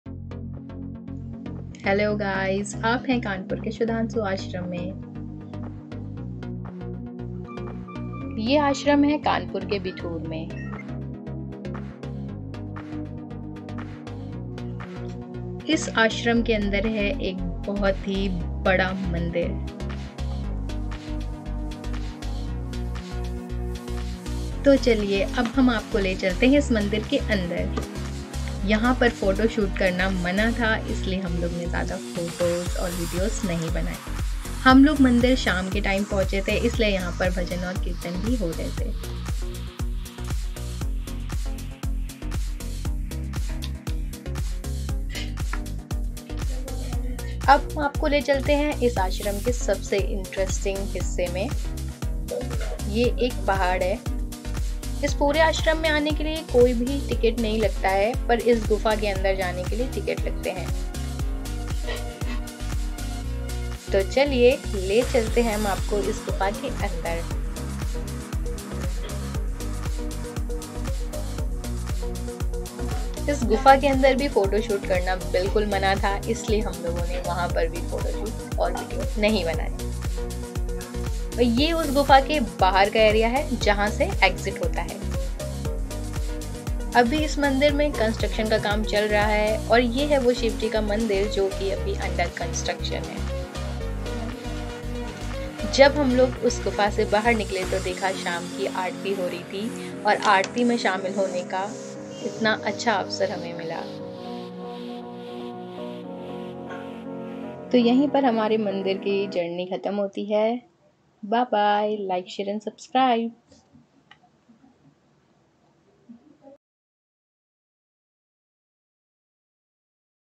Best places to visit in Kanpur, Sudhanshu Ashram
Sudhanshu Ashram Kanpur, This Ashram is on Bithoor Road, Kanpur
This ashram is dedicated to: Lord Krishna, Shivji and Maa Durga.
You can go to the ashram without ticket. 20 rupees ticket has to be taken to go inside the cave. Peace and devotion spread all around & Beautiful gardens here.

Temple timing: 9 AM - 11 AM & 4 PM - 6 PM
Ashram timing:  8 AM - 7 PM